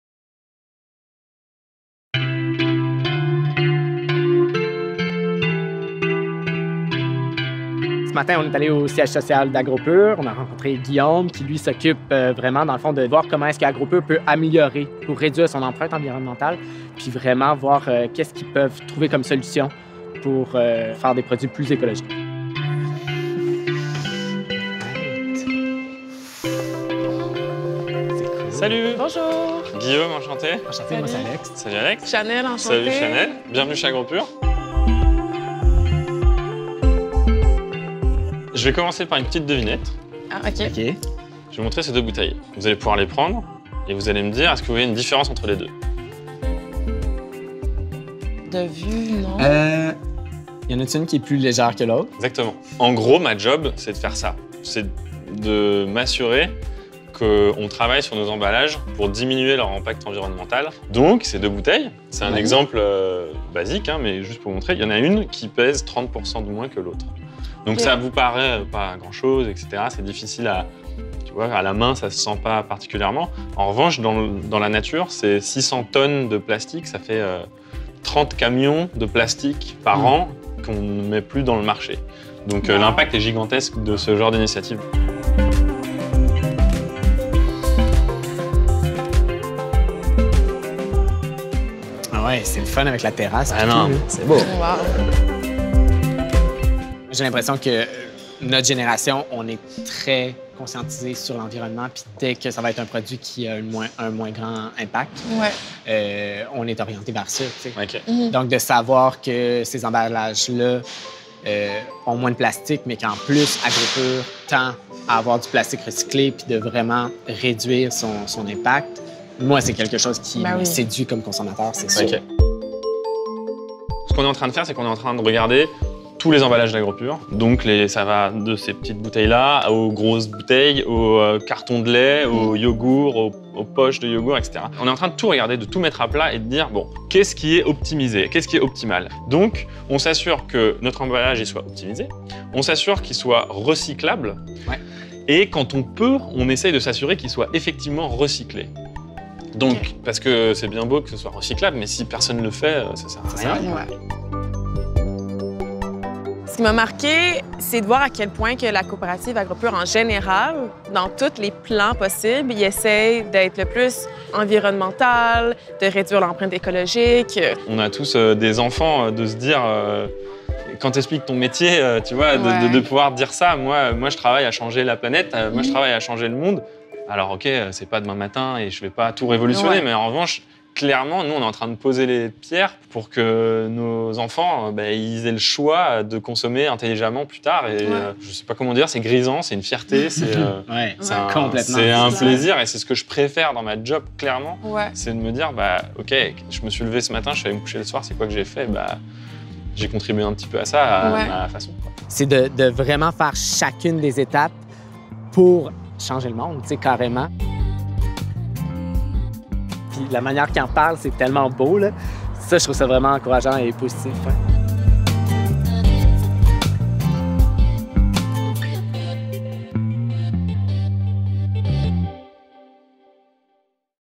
Episode 3: Alex and Channel visit our head office!
For this third episode on packaging, Alex and Channel go to Agropur's head office to meet with Guillaume Stal, sustainable packaging expert. One of Guillaume's accomplishments: the new iögo drinkable yogurt bottles, made of 30% less plastic, which means we are producing 600 tonnes less of plastic per year!

*All the episodes were filmed prior to the COVID-19 pandemic.